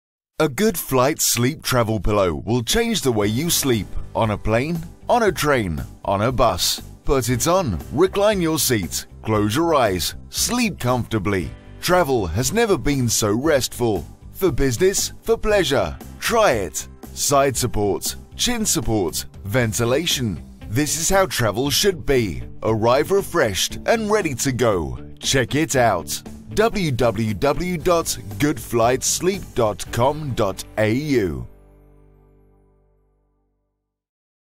Good Flight Sleep video explainer
Have you ever had trouble falling asleep on a plane? Have you ever been jolted awake by your head jerking forward…again? And as a result, you reached your destination tired, stiff and sore?

The goodflightsleep is the solution to your problem.

Hi, I’m Vivienne and back in 2013, a flight from Perth to London was a particular pain in my neck. Despite my best efforts, the relentless head falling meant little sleep. But the worst part was that my stiff, sore neck overshadowed my holiday.

When faced with another long haul flight, I was sure someone would have thought of a way to avoid this dilemma. But after hours of searching, I came up empty handed. That’s why I decided to solve the problem myself.

The goodflightsleep supports your head on the sides but what really sets it apart from other neck pillows is that it supports your chin too.

This prevents your head from lolling in any direction as you sleep and the holes provide ventilation so your neck doesn't get too hot. So you can enjoy a relaxed, comfortable flight.

After many test flights and design improvements the final design is patented and available now.

Make your next flight an enjoyable one…Happy sleeping.